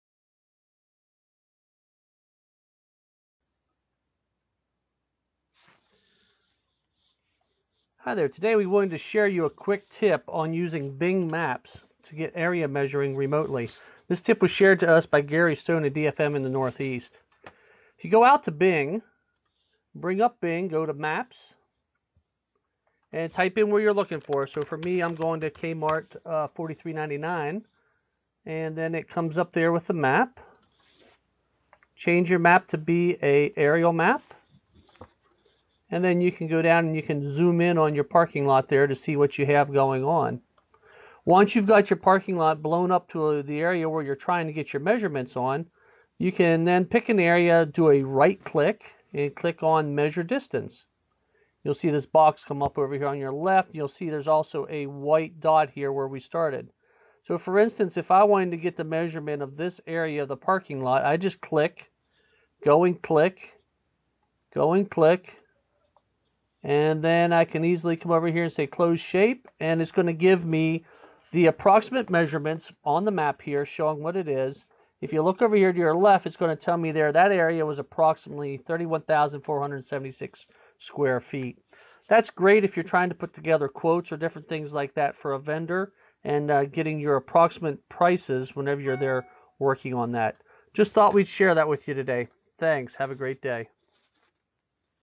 Bing Map Area Measuring 2017
Using the Bing.com Satellite map to get approximate measurements to get bids or future project numbers.